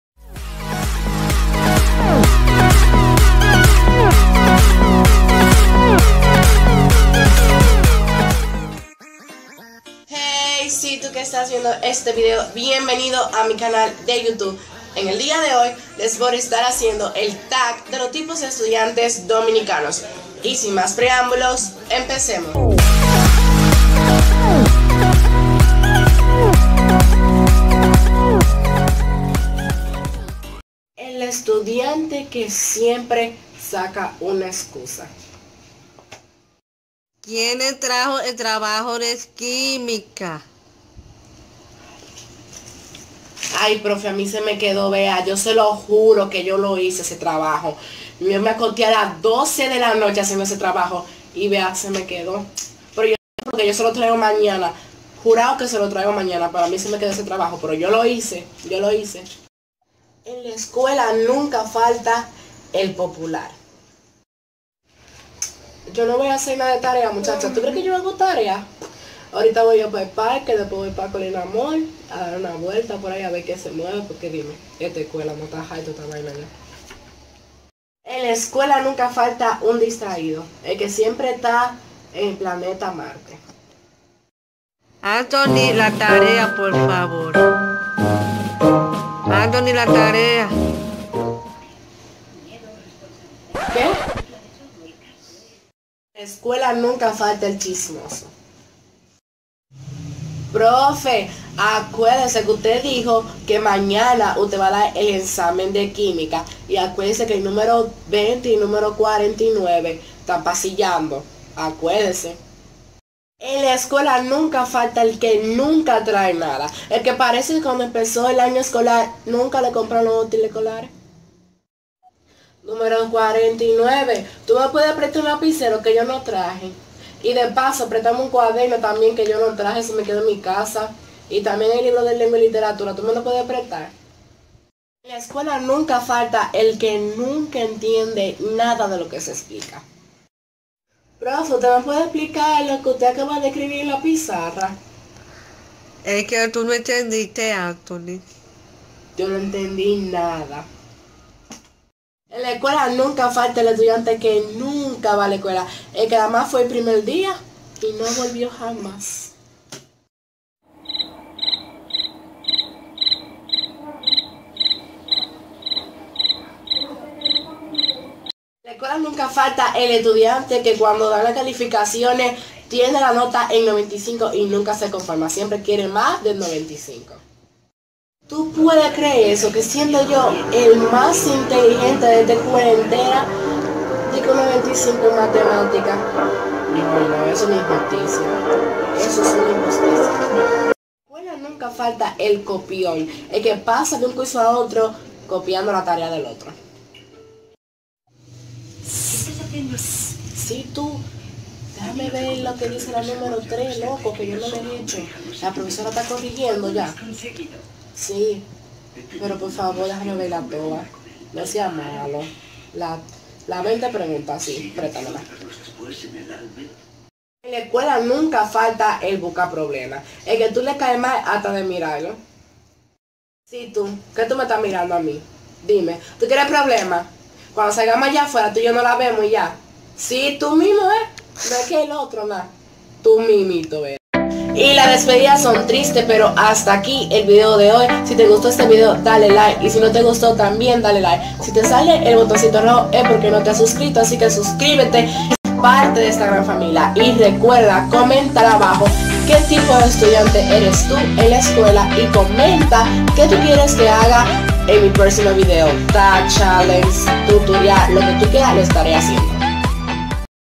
TAG| Tipos De Estudiantes DOMINICANOS| Tú cuál eres? |Anthony Music RD
Hey! Si tu que estas viendo este vídeo, BIENVENIDO/A !! A mi canal de youtube!
Les Estaré Haciendo El Tag De Los Tipos De Estudiantes DOMINICANOS